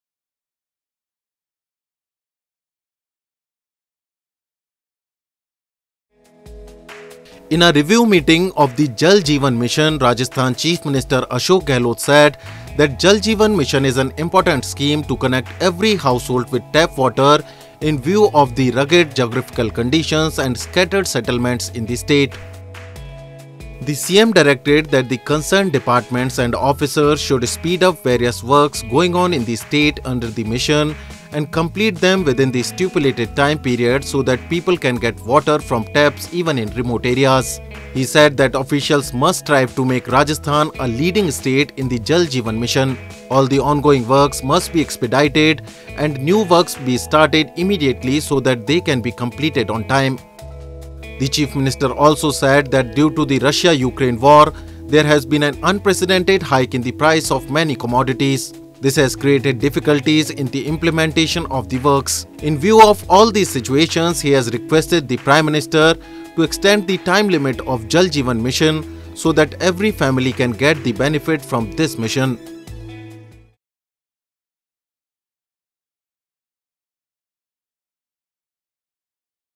Complete Jal Jeevan Mission work on time: CM Gehlot
Jaipur (Rajasthan) June 18 (ANI): In a review meeting of the Jal Jeevan Mission, Rajasthan Chief Minister Ashok Gehlot said that Jal Jeevan Mission is an important scheme to connect every household with tap water in view of the rugged geographical conditions and scattered settlements in the State. The CM directed that the concerned departments and officers should speed up various works going on in the State under the mission and complete them within the stipulated time period so that people can get water from taps even in remote areas. He said that officials must strive to make Rajasthan a leading state in the Jal Jeevan Mission. All the ongoing works must be expedited and new works be started immediately so that they can be completed on time.The Chief Minister also said that due to the Russia-Ukraine war, there has been an unprecedented hike in prices of many commodities. This has created difficulties in the implementation of the works. In view of all these situations, he has requested the Prime Minister to extend the time limit of Jal Jeevan Mission, so that every family can get the benefit from this mission. (HR)